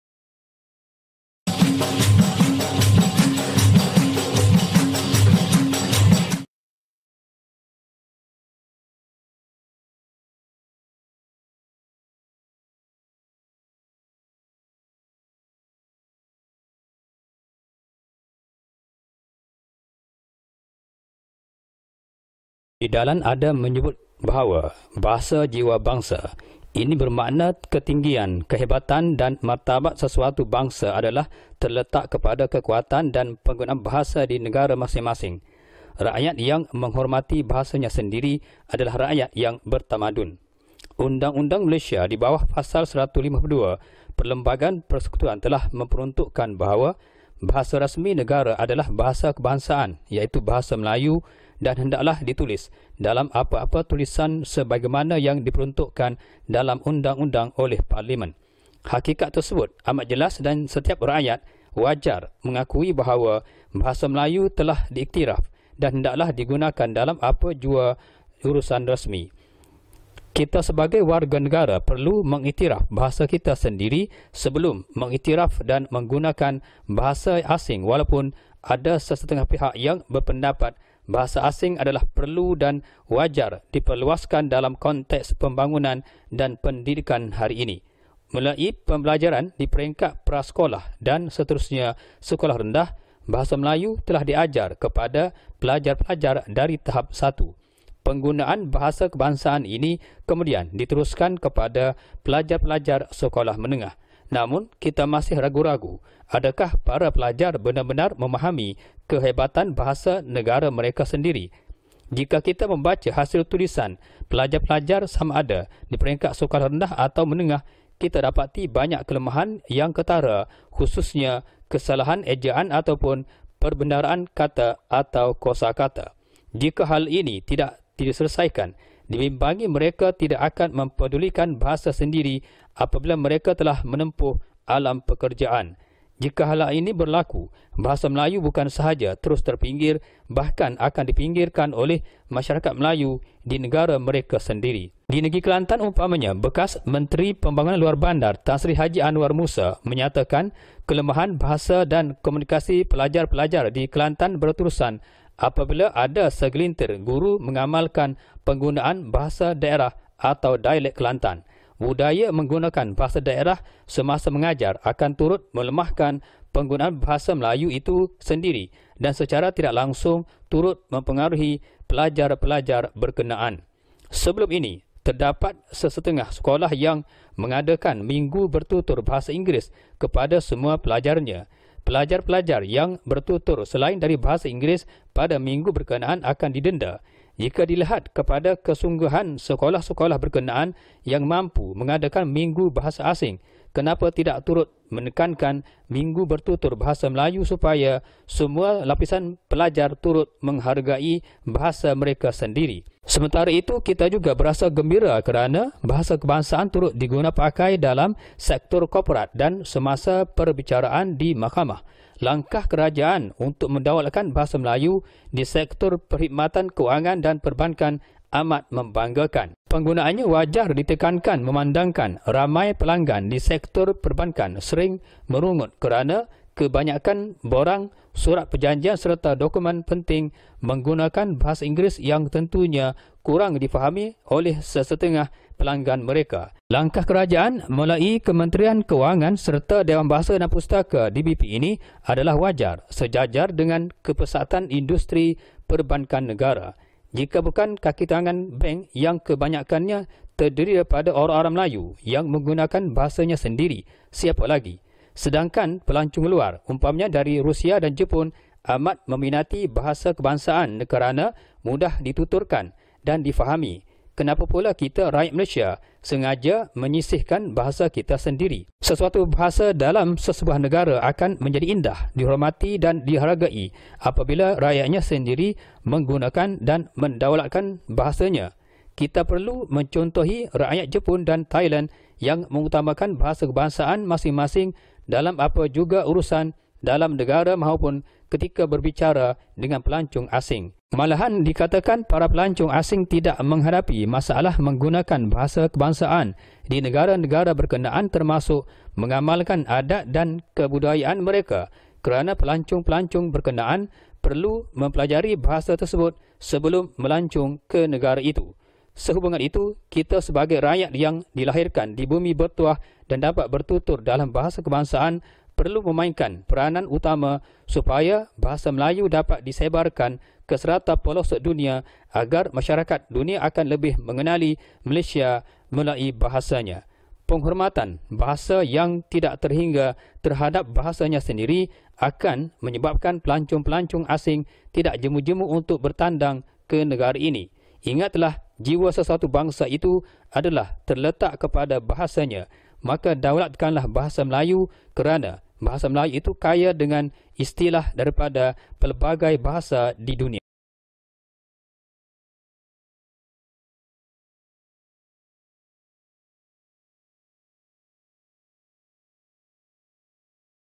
Bahasa Jiwa Bangsa
Video dan Rencana membicarakan tentang ketinggian dan martabat bahasa Melayu yang perlu dipertahankan dan terus digunakan sebagai bahasa peradaban dan dunia. Bahasa menunjukkan jati diri dan jiwa sesuatu bangsa itu.